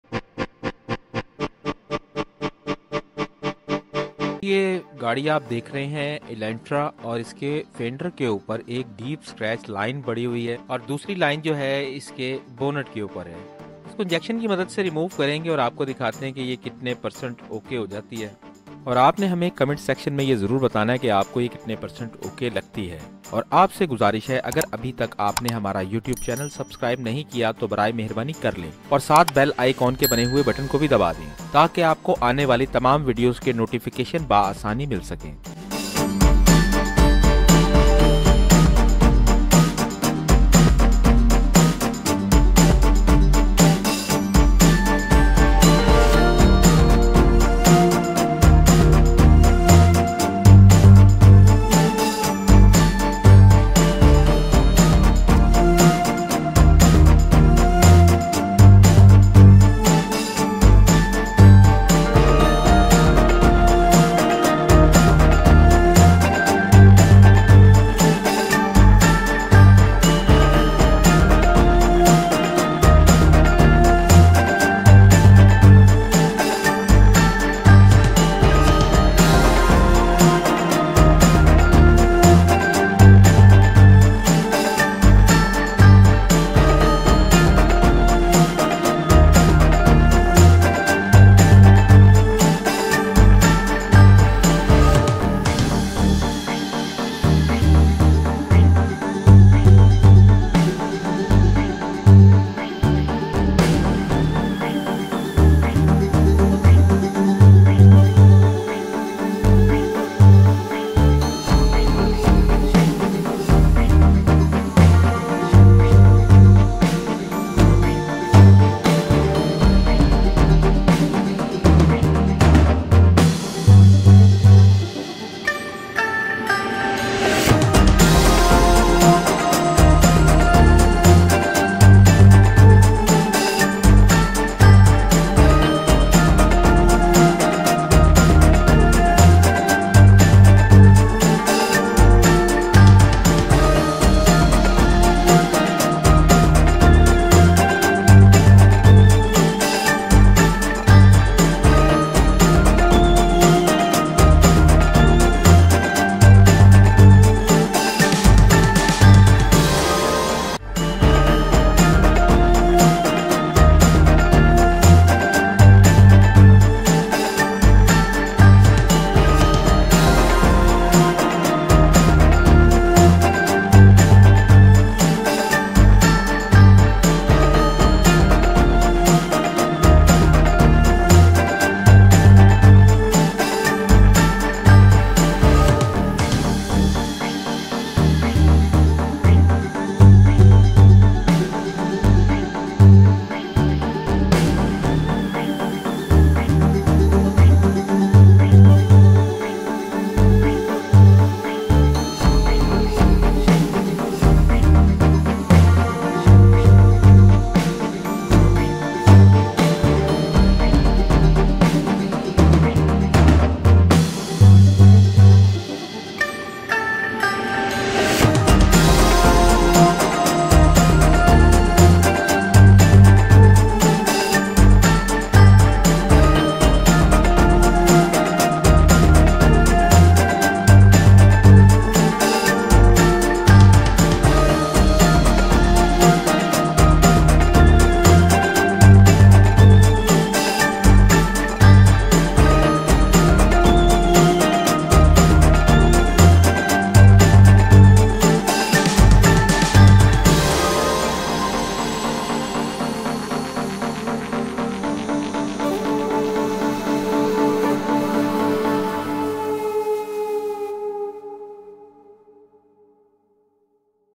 Hyundai Elantra Front front Fender And Bonnet Deep Scratch Removing without paint / mateen auto
Mateen Auto Workshop Best Denting Painting Work
Specialist Paintless Scratch Remover Work
Address: H824+RQH, Bahawalpur Rd, Mozang Chungi, Lahore, Punjab 54000